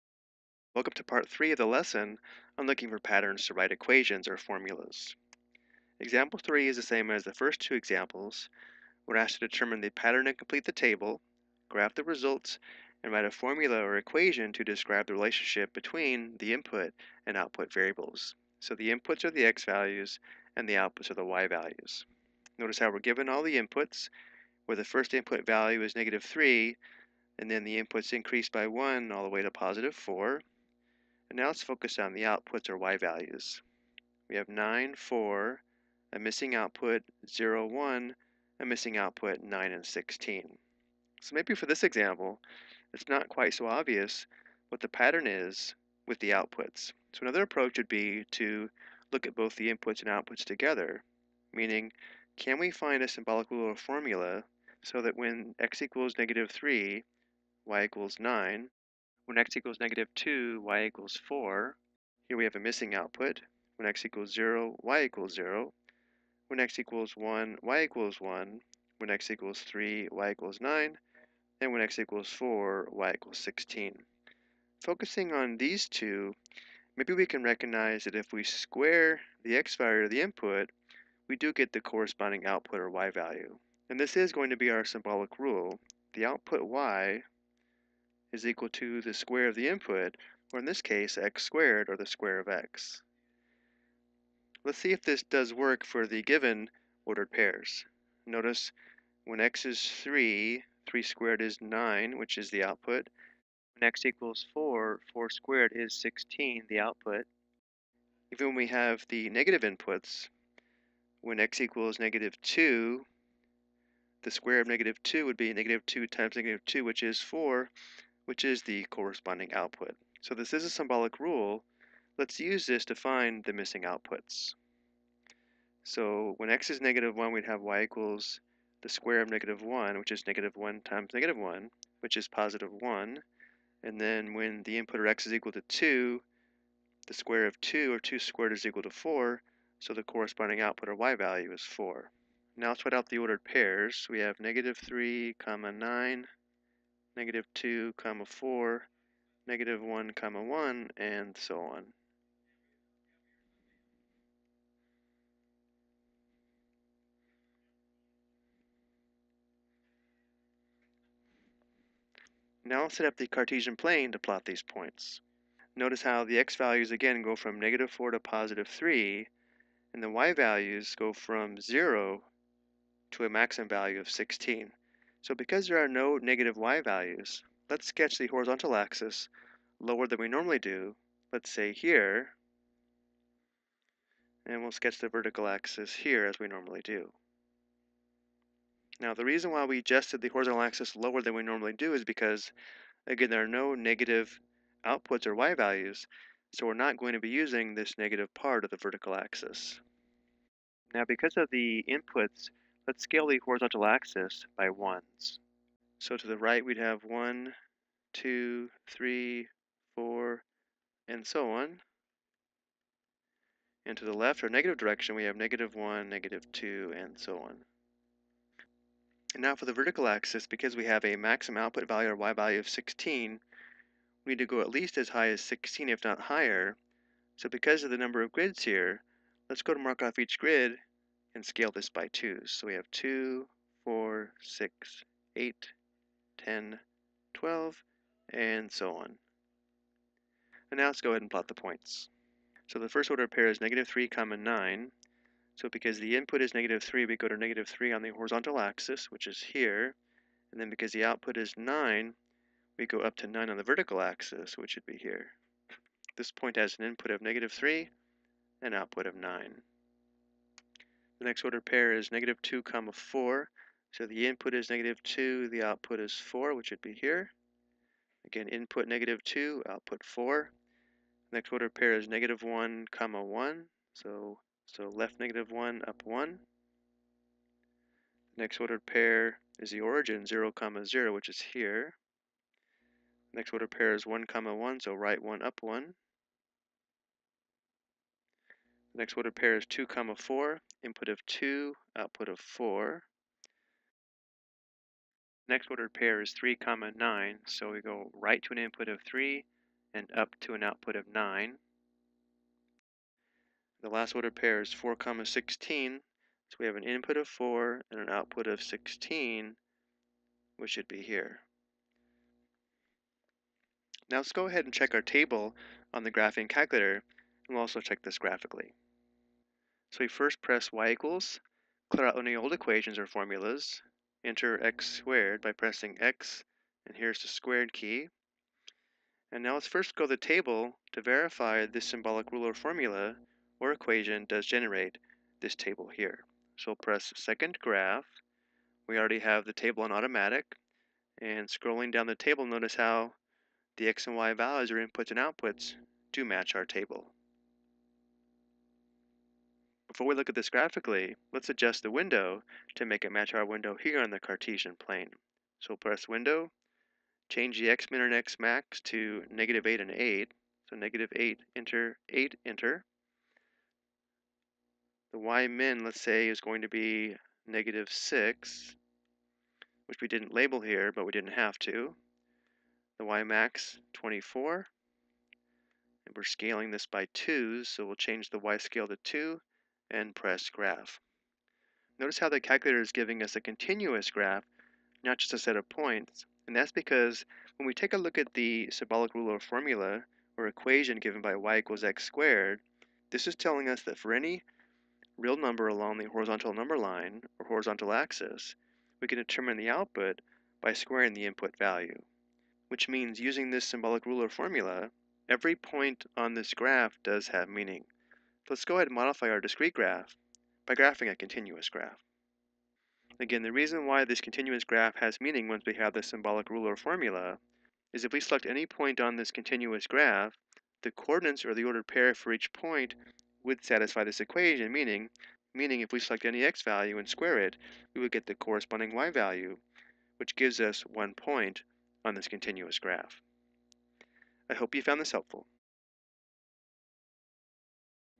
Writing Equations From Tables - Squaring (L6.6)
This video lesson explains how to complete a table of values by recognizing a pattern, write an equation for the table rule, and plot the points on the Cartesian plane.

Content created by Jenifer Bohart and Amy Volpe from Scottsdale CC (License CC-BY-SA 4.0)